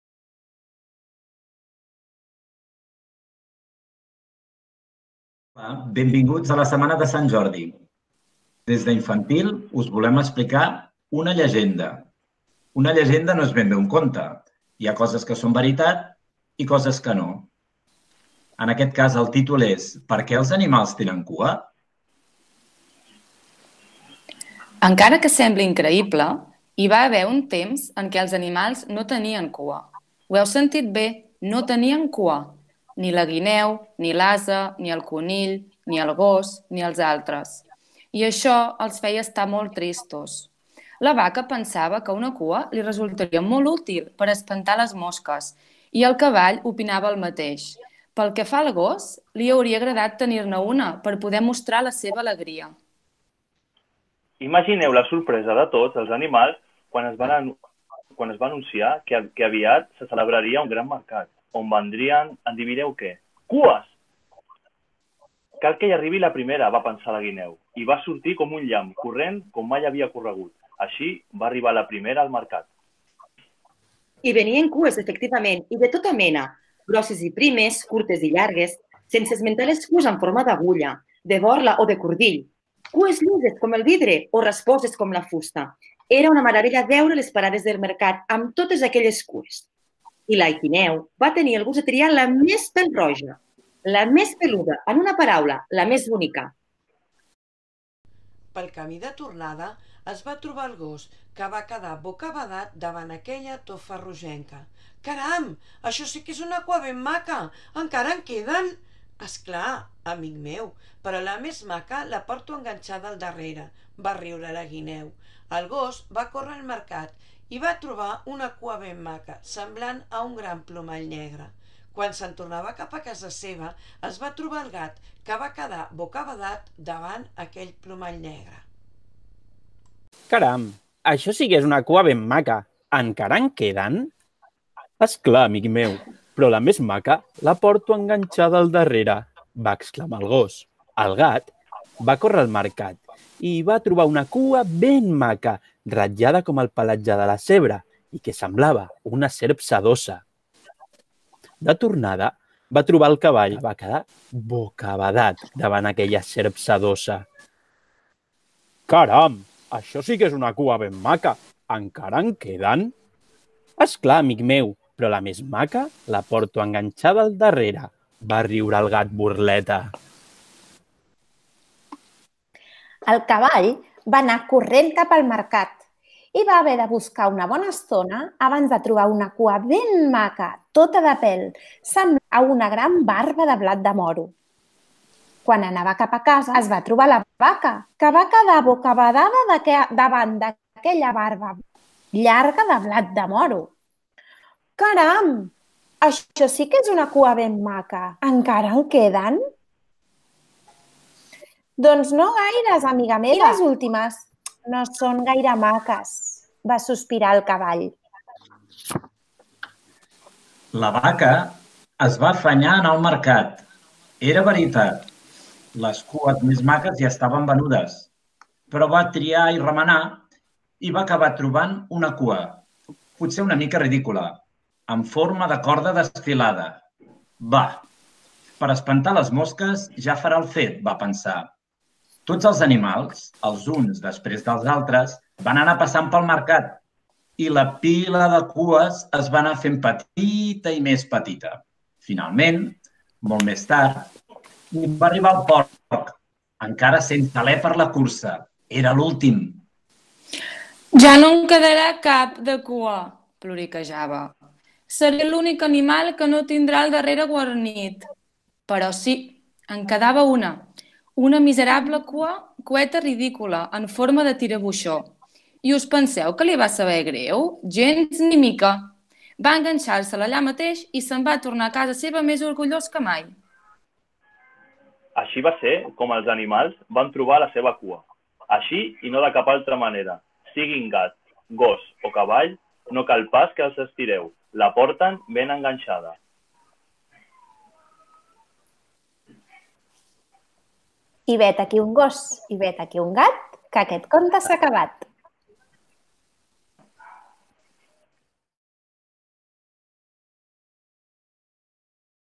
Sant Jordi amb l'equip d'Infantil de Nou Patufet
L'equip d'Infantil de NouPatufet també ha fet un vídeo col·lectiu on expliquen als més petits la llegenda "Per què els animals tenen cua"